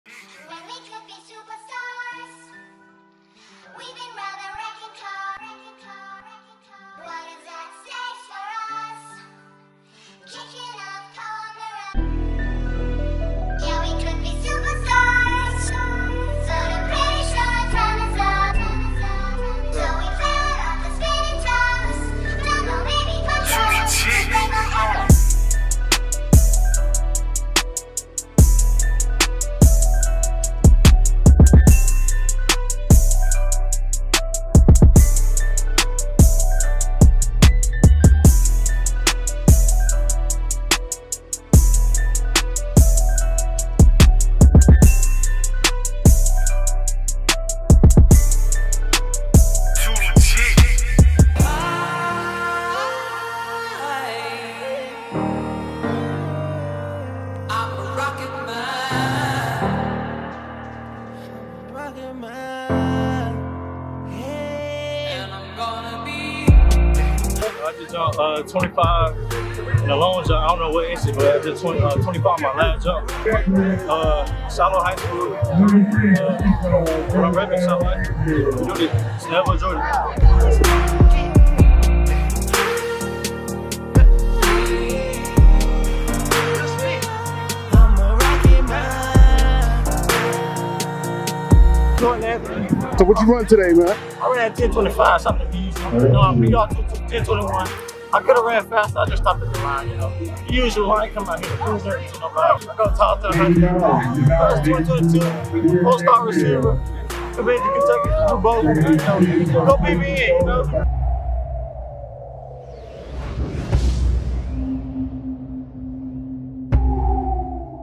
Westlake High School Christian Coleman Track & Field 2021 Invitational pt 1
Live from Westlake High School in Atlanta, GA.  we caught some great images from one of the best High School Track and Field Meets in the country.